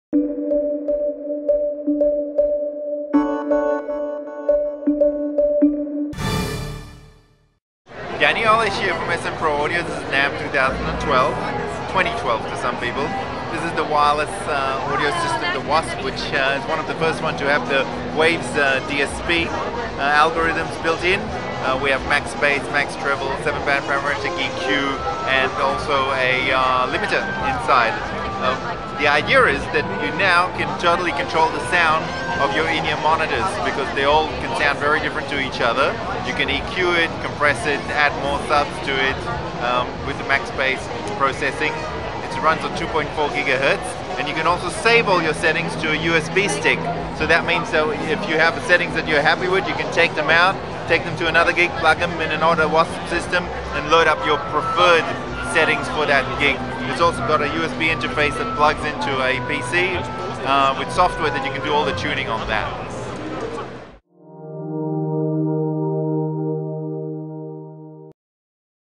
SM Pro Audio W.A.S.P.
The W.A.S.P. is the first in-ear monitoring system to use  WAVES algorithms to control and enhance the sound of personal monitors. It's also the first in-ear monitoring system to include full control over USB for all DSP parameters and the ability to save your favorite setup presets onto USB stick, so you can re-load them into the system no matter where you are.

W.A.S.P. has two modes, Master and Slave, in Master Mode USB Flash drives can be used, while in Slave mode the unit can be connected to a computer, and all the DSP parameters tuned and saved via the supplied software.